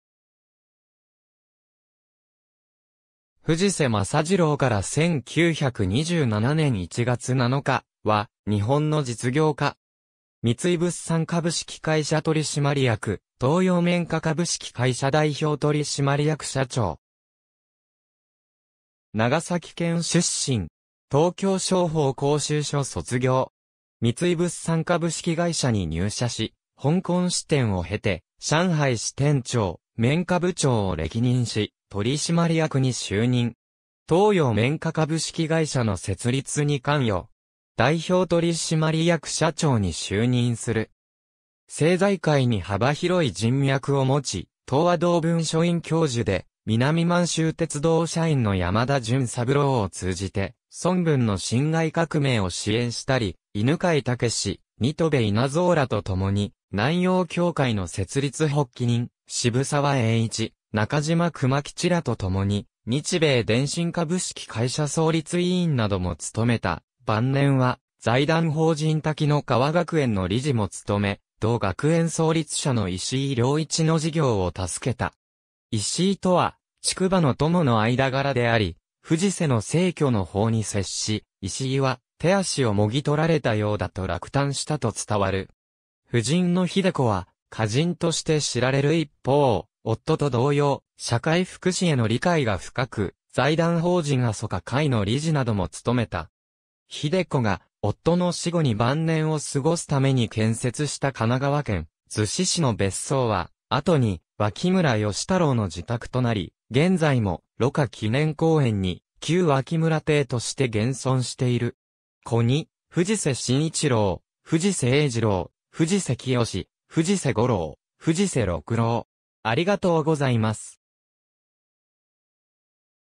藤瀬政次郎
藤瀬 政次郎（ふじせ まさじろう、1867年2月9日（慶応3年1月5日） - 1927年（昭和2年）1月7日）は、日本の実業家。
三井物産株式会社取締役、東洋綿花株式会社（後のトーメン、現在の豊田通商）代表取締役社長。
長崎県出身。
東京商法講習所（現在の一橋大学）卒業。
三井物産株式会社に入社し、香港支店を経て、上海支店長、綿花部長を歴任し、取締役に就任。
東洋綿花株式会社の設立に関与（同社の設立は、三井物産綿花部が分離独立したもの）。
代表取締役社長に就任する。
政財界に幅広い人脈を持ち、東亜同文書院教授で、南満州鉄道社員の山田純三郎を通じて、孫文の辛亥革命を支援したり、犬養毅、新渡戸稲造らとともに南洋協会の設立発起人、渋沢栄一、中島久万吉らとともに日米電信株式会社創立委員などもつとめた、晩年は、財団法人滝乃川学園の理事もつとめ、同学園創立者の石井亮一の事業を援けた。
石井とは「竹馬の友」の間柄であり、藤瀬の逝去の報に接し、石井は「手足をもぎ取られたようだ」と落胆したと伝わる。
夫人の秀子は、歌人として知られる一方、夫と同様、社会福祉への理解が深く、財団法人あそか会の理事などもつとめた。
秀子が夫の死後に晩年を過ごすために建設した神奈川県逗子市の別荘は、後に脇村義太郎の自宅となり、現在も蘆花記念公園に「旧脇村邸」として現存している（非公開）。
子に藤瀬新一郎（東北大学教授、妻孝子は林博太郎次女）、藤瀬英二郎（日本貿易振興会理事、妻信子は益田太郎長女）、藤瀬清、藤瀬五郎（NHK解説委員、妻百代は小池銀行頭取小池国三の娘）、藤瀬六郎。